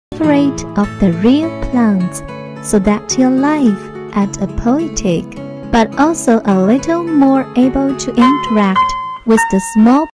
Music Plants Flower Pot
Smart Touch Plant Piano Music Playing Light Round Pot, Night Light Wireless Flowerpot with Bluetooth speaker